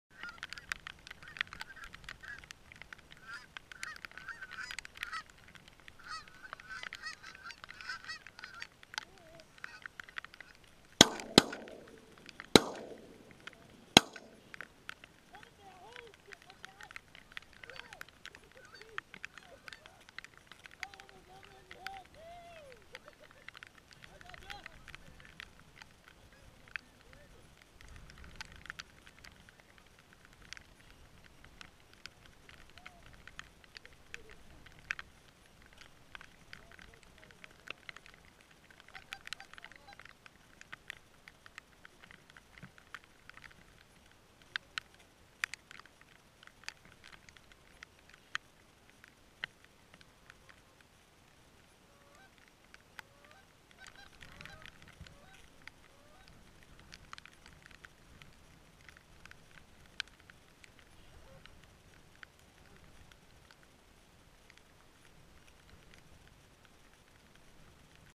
Geese in the fog
Goose hunting on a foggy morning on Maryland last season